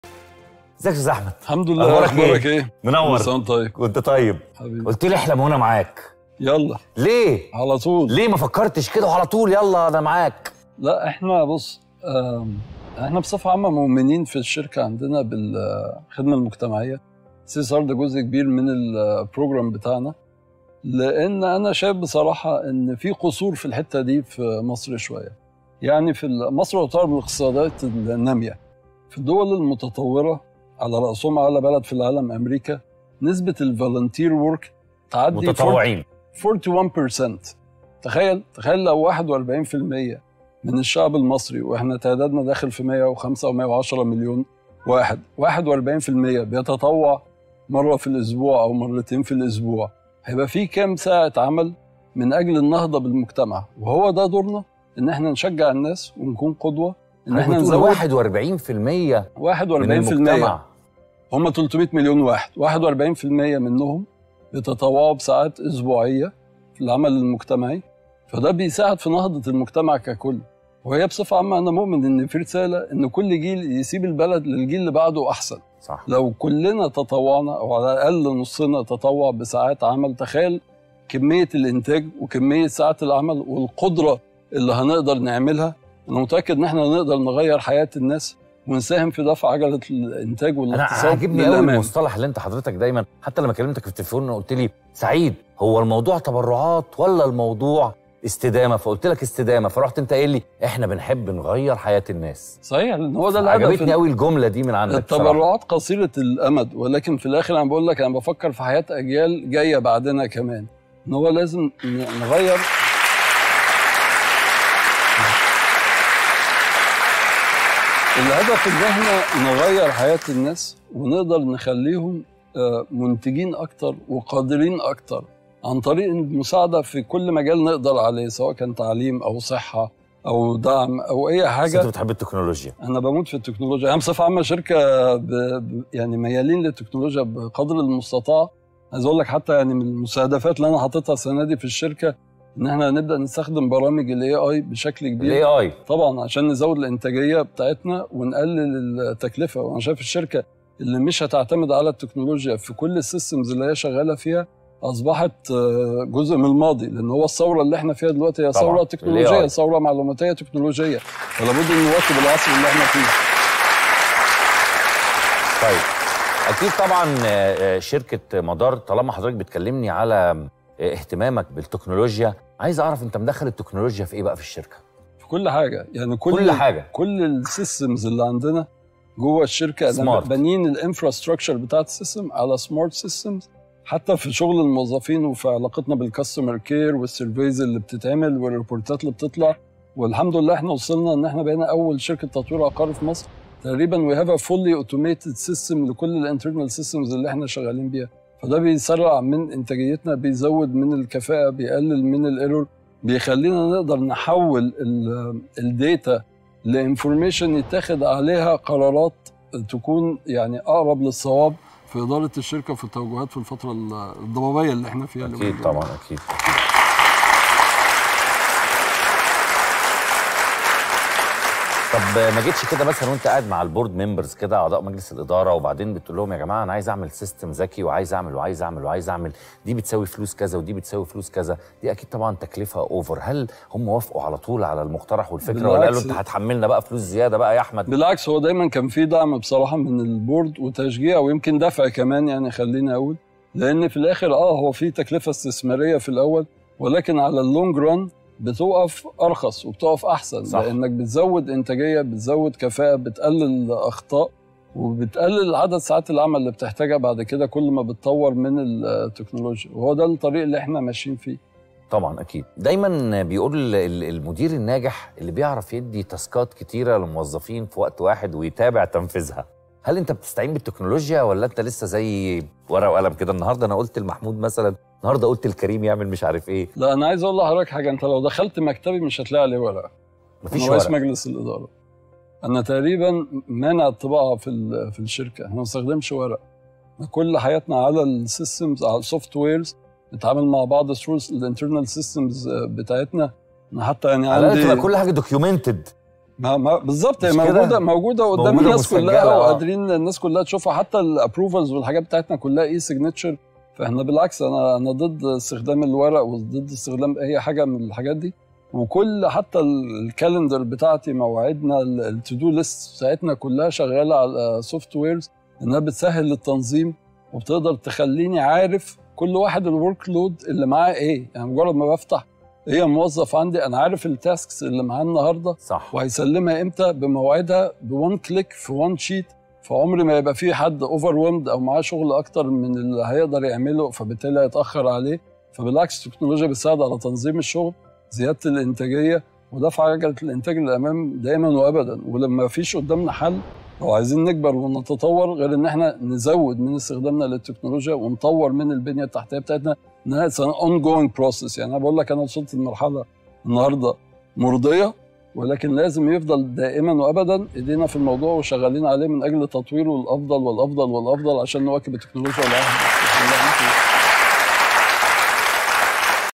الرئيس التنفيذي لشركة مدار للتطوير العقاري م.احمد ايهاب: الخدمة المجتمعية تشكل جزء كبير من برنامجنا
الرئيس التنفيذي لشركة مدار للتطوير العقاري م.احمد ايهاب: الخدمة المجتمعية تشكل جزء كبير من برنامجنا ونؤمن بأهميتها